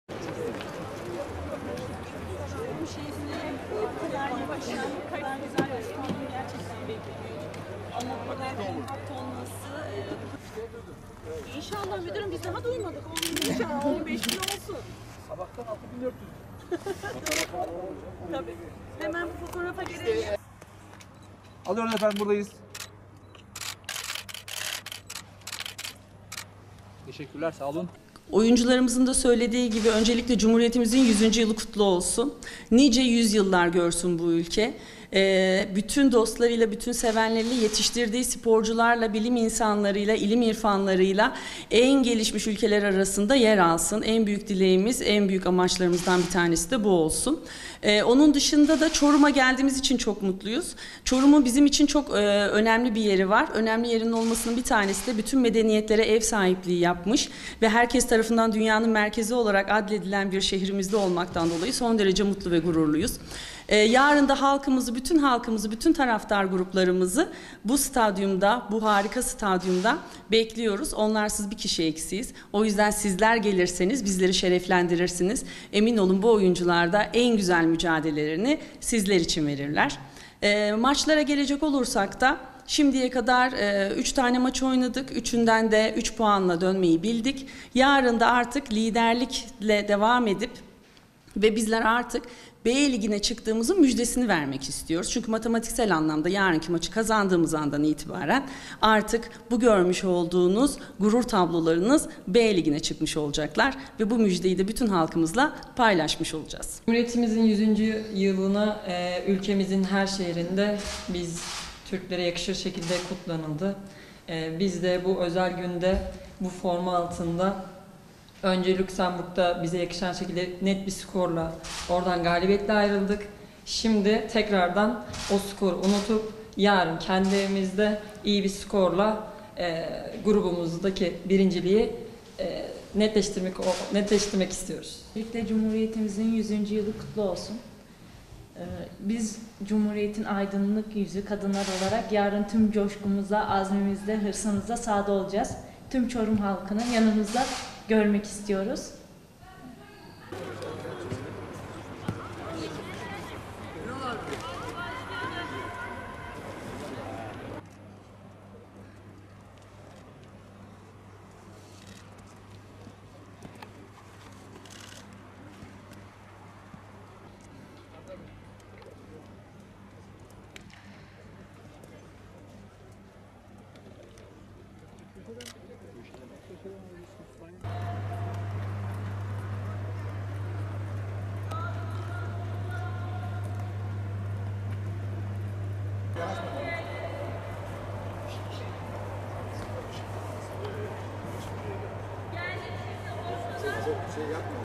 Necla Güngör Kıragası: B ligine çıkışımızın müjdesini vermek...
UEFA Kadınlar Uluslar Ligi’nde mücadele eden A Milli Kadın Futbol Takımı, Lüksemburg maçı öncesi son antrenmanını maçın oynanacağı Çorum Şehir Stadyumu’nda yaptı. Teknik Direktör Necla Güngör Kıragası, "Yarın lider olup B ligine çıkışımızın müjdesini vermek istiyoruz" dedi.